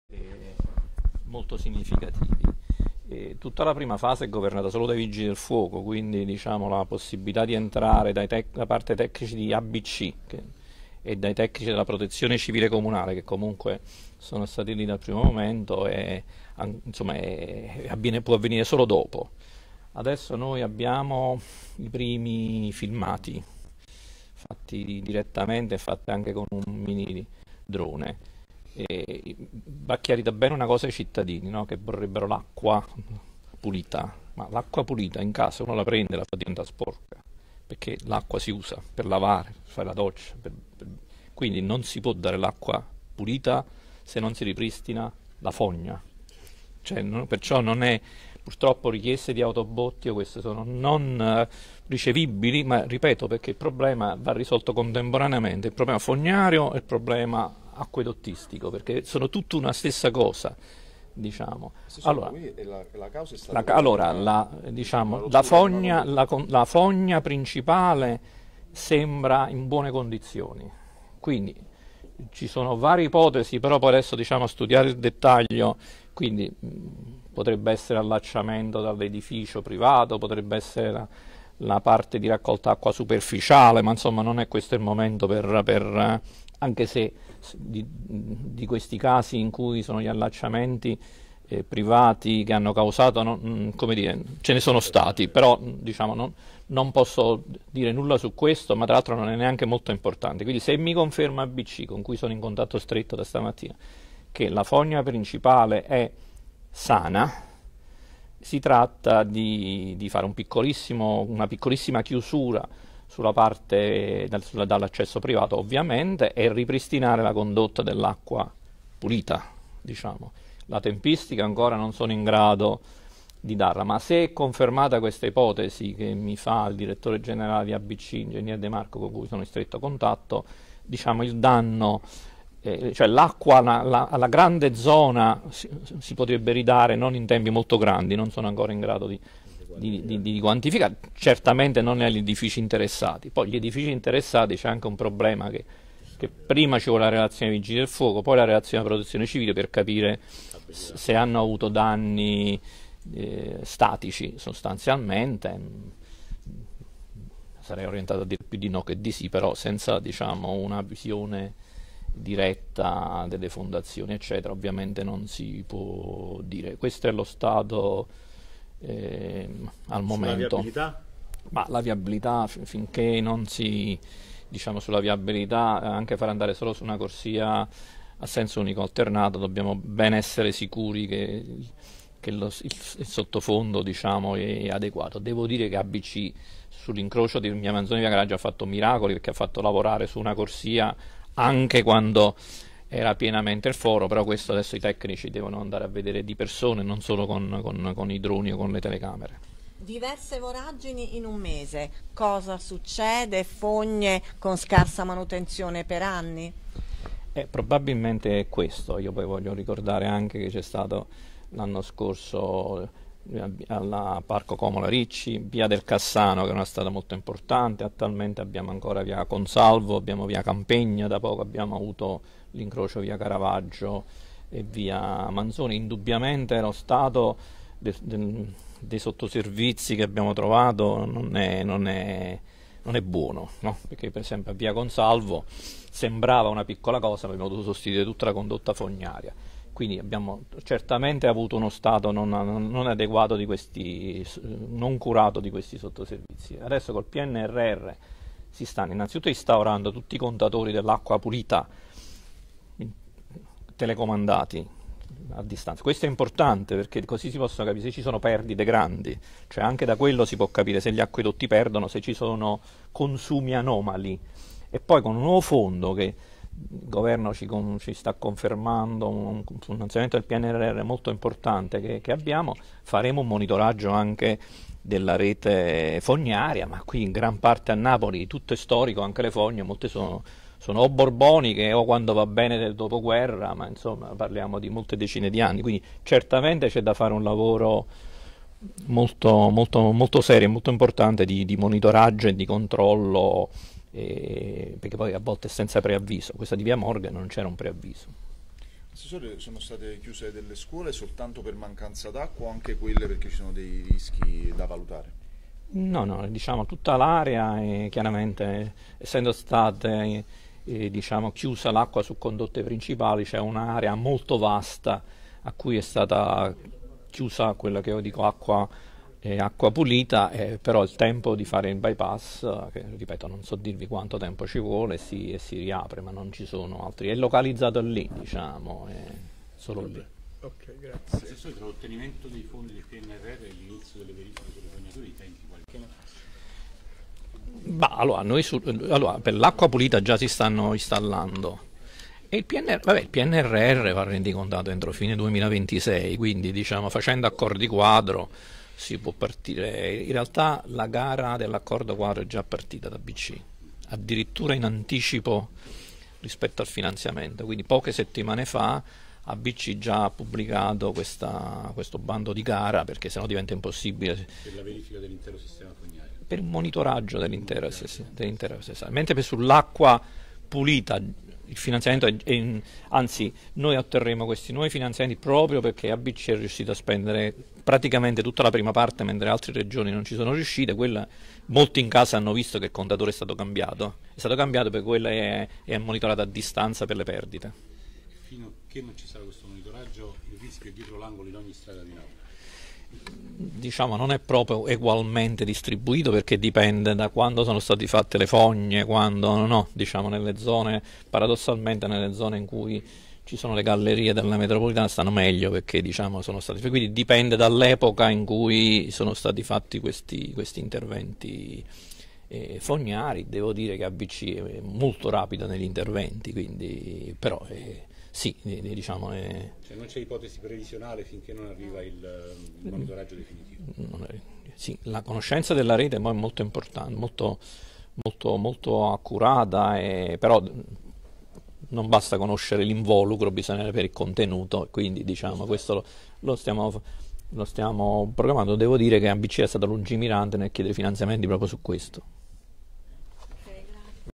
Le parole dell’Assessore Edoardo Cosenza sulla voragine Via Morghen al Vomero
Live da SalaGiunta
Le parole dell’Assessore EdoardoCosenza sulla voragine Via Morghen al Vomero